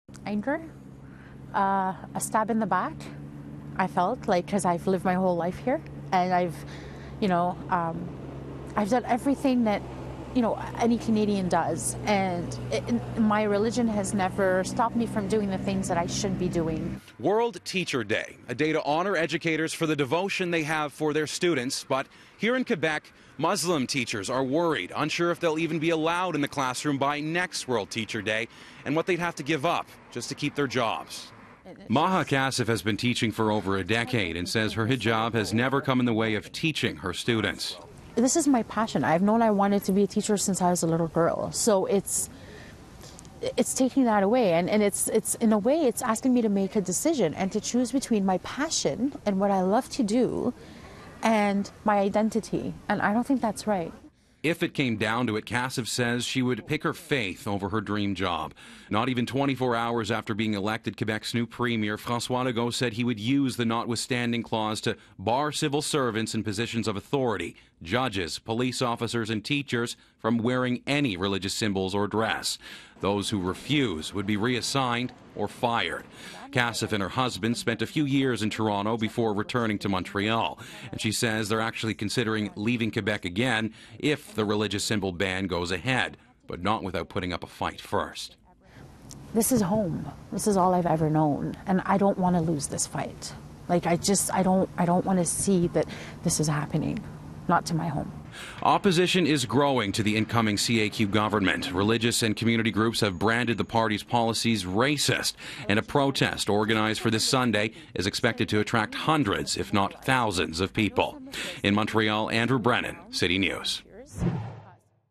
Muslim teacher fearful over religious symbol ban
A Montreal teacher says she is angry over a proposed ban on religious symbols—and afraid she'll be forced to choose between her job and her faith.